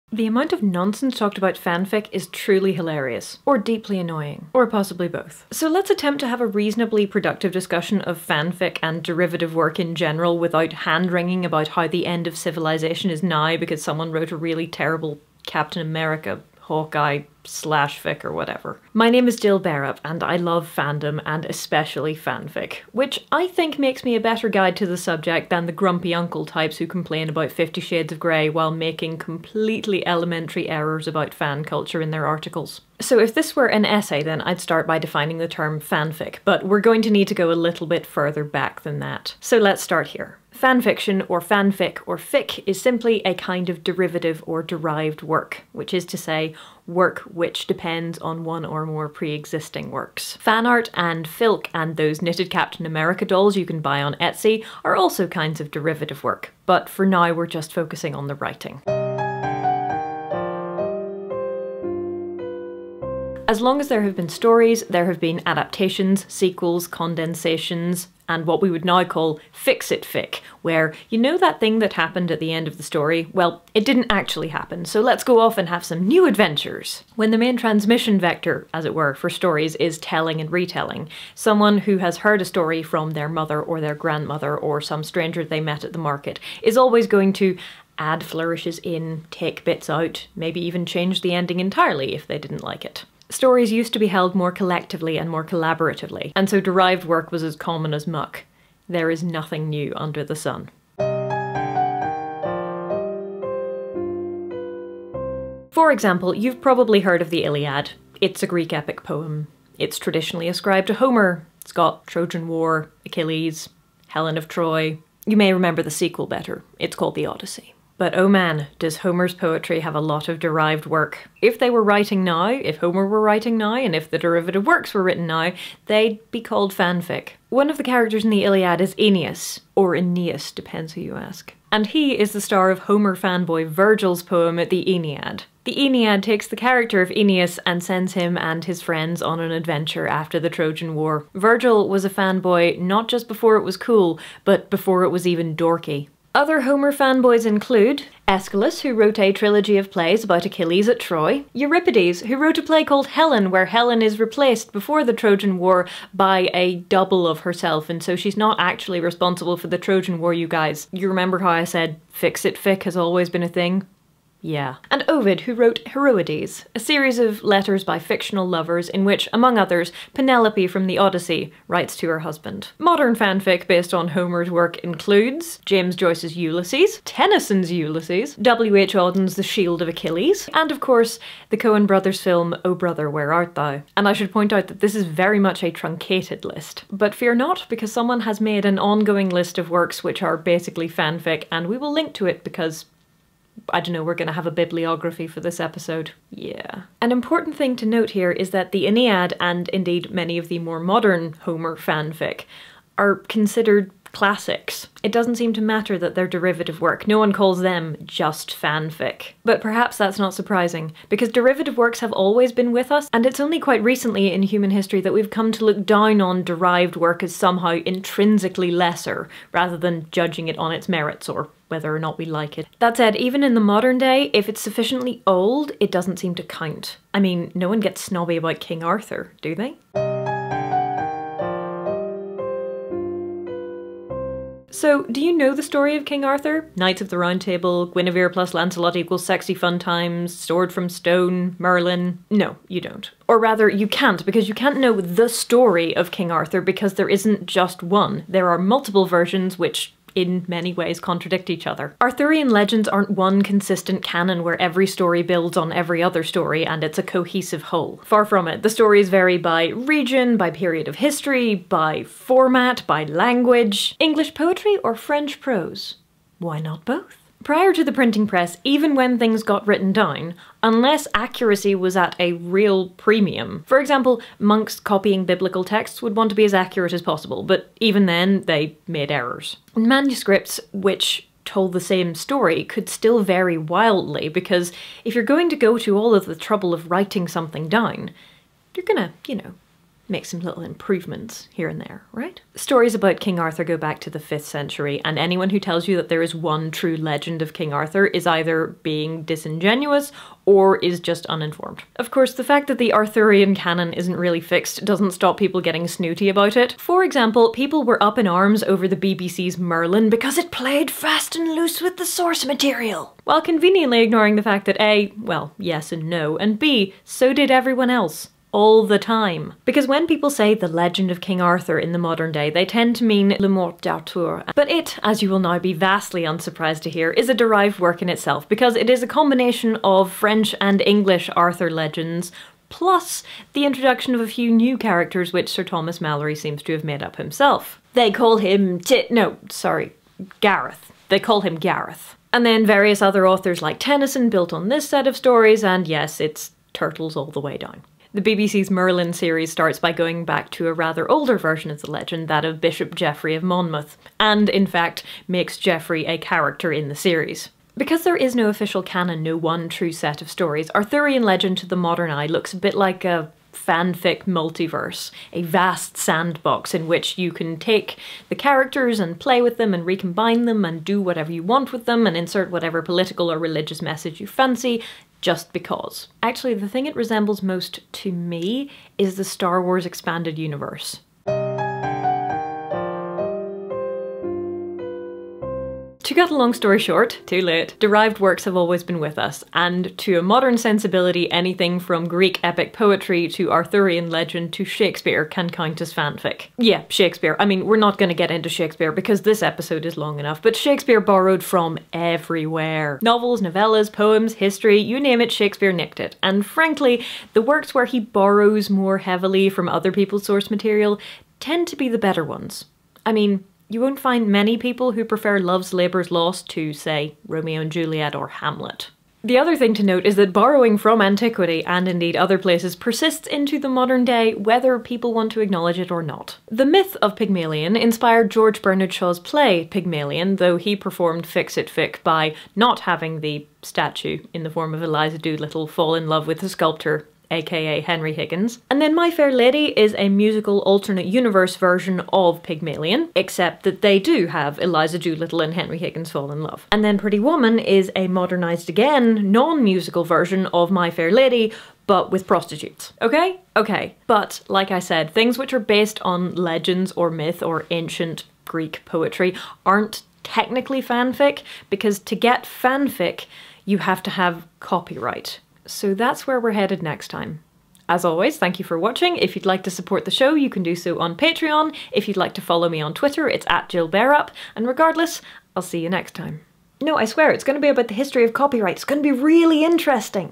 Virgil was a Homer Fanboy | History of Fanfiction 1
In the first part of our series on the history of fanfiction, we discuss derivative works, Ancient Greek poetry, Arthurian legend and how Virgil was a Homer fanboy not just before it was cool, but before it was even dorky.